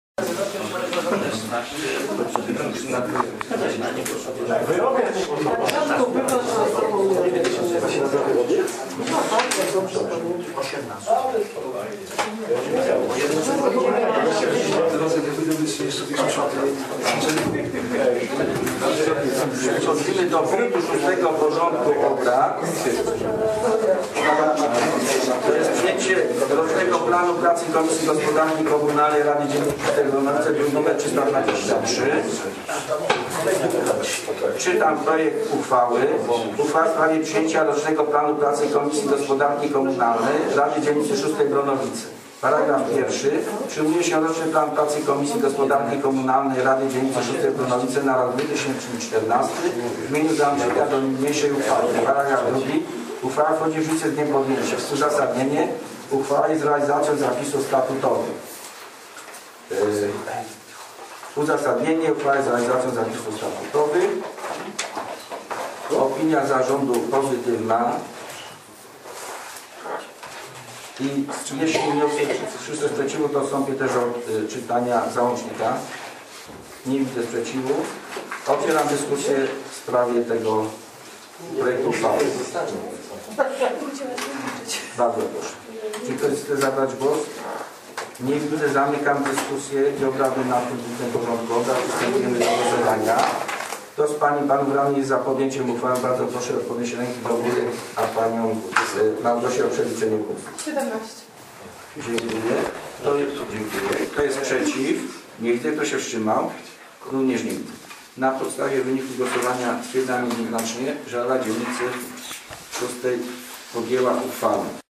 RD_VI_13-02-2014_6 przyjęcie rocznego planu pracy Komisji Gospodarki Komunalnej
Obrady XXXIX sesji Rady Dzielnicy VI Bronowice z dn. 13 lutego 2014 r. Uchwała w sprawie przyjęcia rocznego planu pracy Komisji Gospodarki Komunalnej Rady Dzielnicy VI Bronowice.